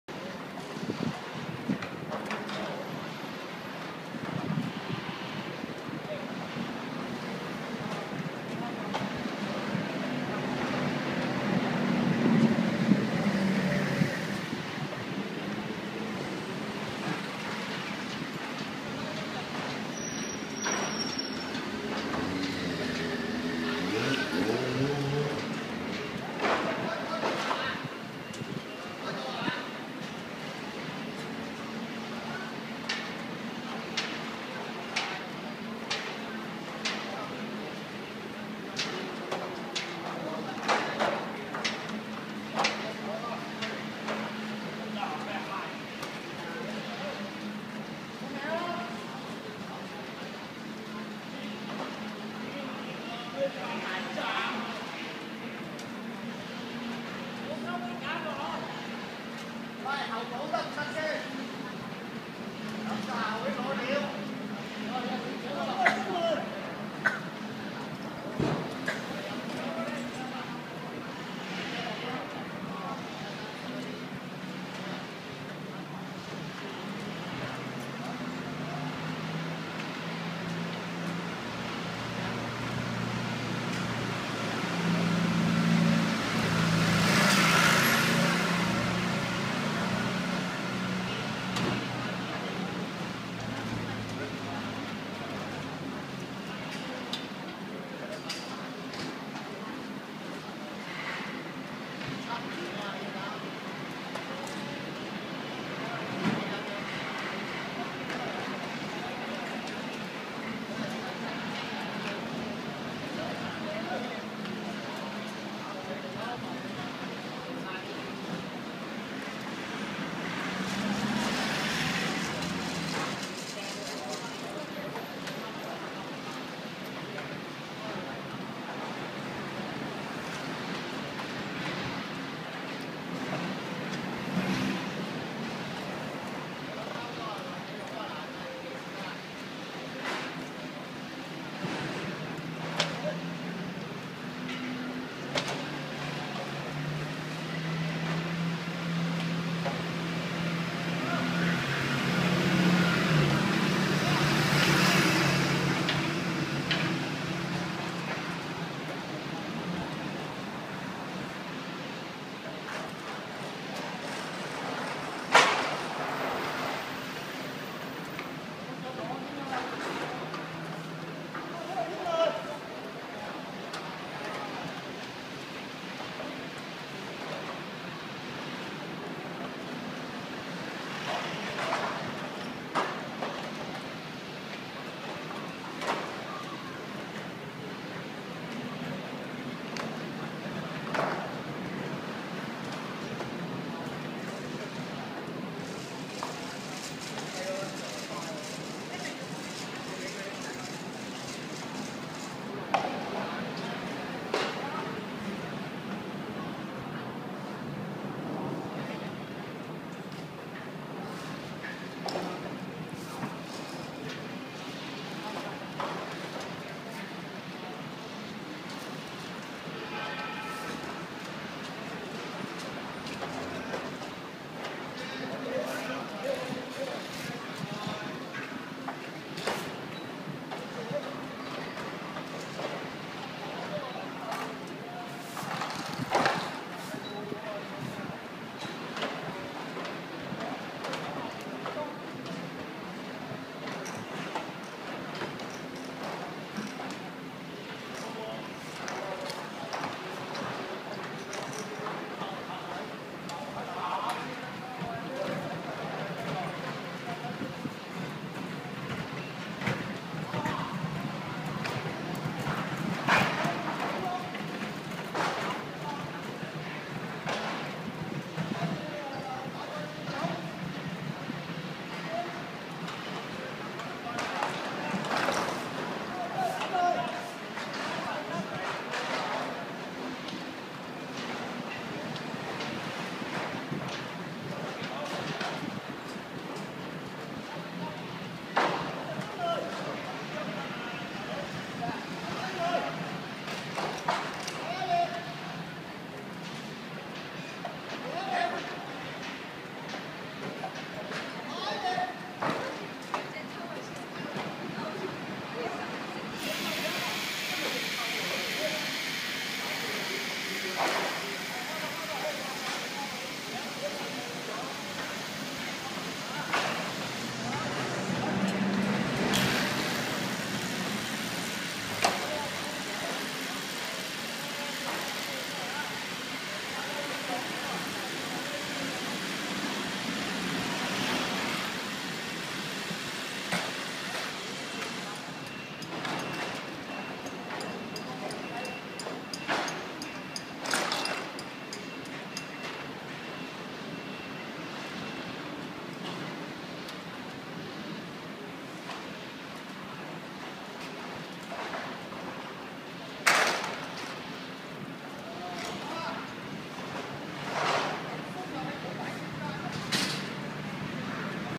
香港名物！工事現場の竹で組む足場。
香港の街角で見かける建築工事。その足場は、日本だと鉄パイプや鉄板を組み立てたものを使いますが、香港では竹を縦横にナイロンのひもで縛って組んでいきます。超高層ビルでも同様で、建物の周りを竹がぐるりと取り囲みます。

その組立は、既に組まれている竹の足場に職人さんがのぼり、長さ７、８メートルもある竹をナイロン製のヒモで次々とくくり付けていくだけです。

そんな簡単で倒れてこないのか？安全なのか？大丈夫か？と思ってしまいますが、竹の足場(竹棚)職人には専門の職業訓練校があり、一定の技術習得を修め、実習も積まないと、竹の足場職人にはなれません。ちなみに、竹の足場職人は他の一般建築作業員に比べて高い賃金であり、人気の職業だそうです。

そんな竹の足場を組んでいる現場に通りがかり、食い入るように職人さんの動きを見てしまいました。